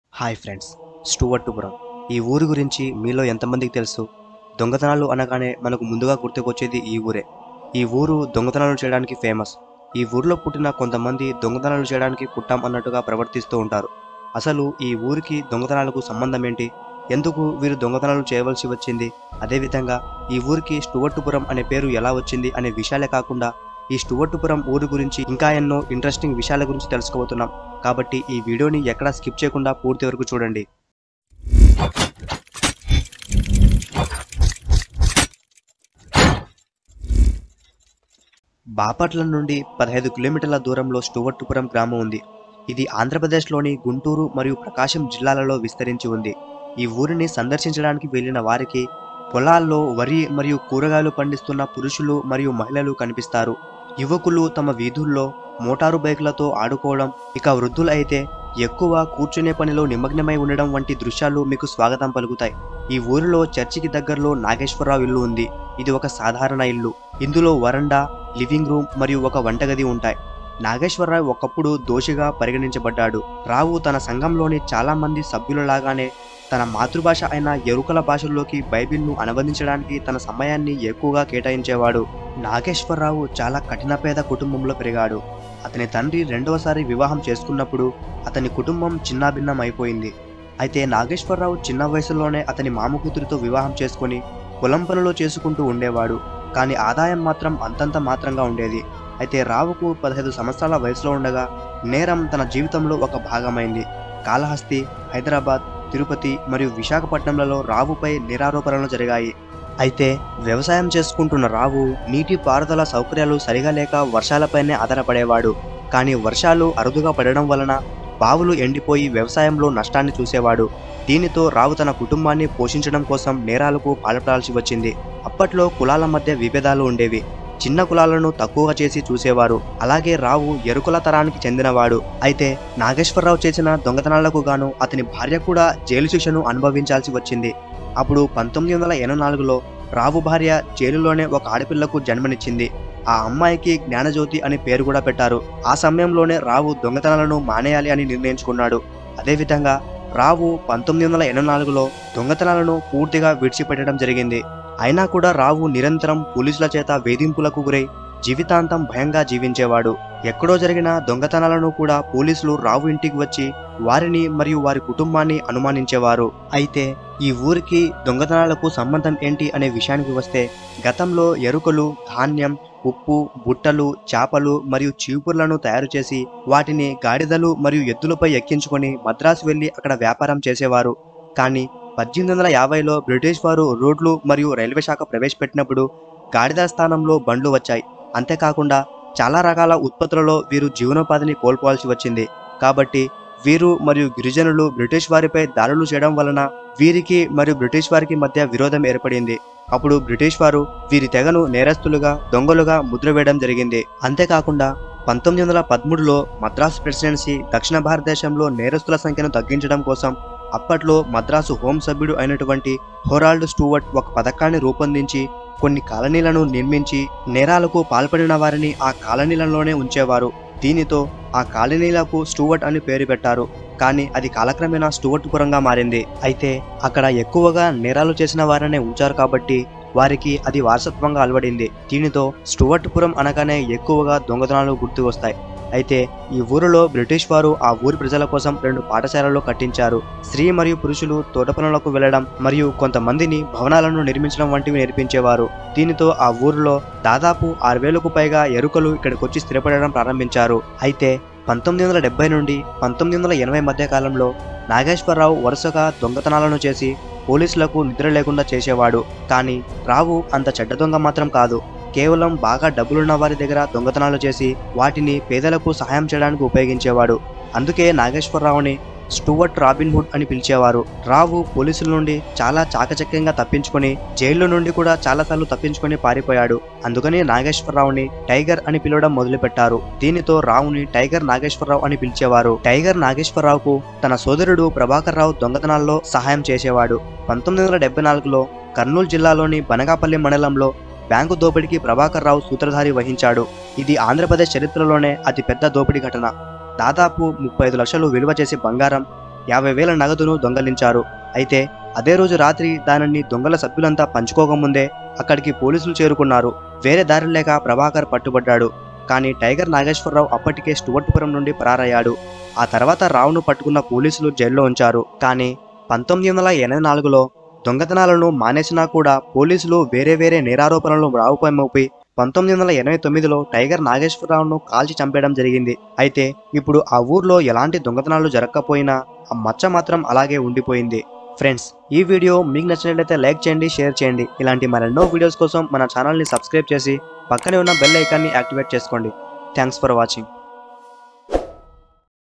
Who Is Tiger Nageswara Rao | Tiger Nageswara Rao Real Story |Stuvartpuram | Theory Of History Telugu
Who Is Tiger Nageswara Rao | Tiger Nageswara Rao Real Story In Telugu | Stuvartpuram | Theory Of History Telugu

Historical Topics, Interesting Facts And Mysterious Stories...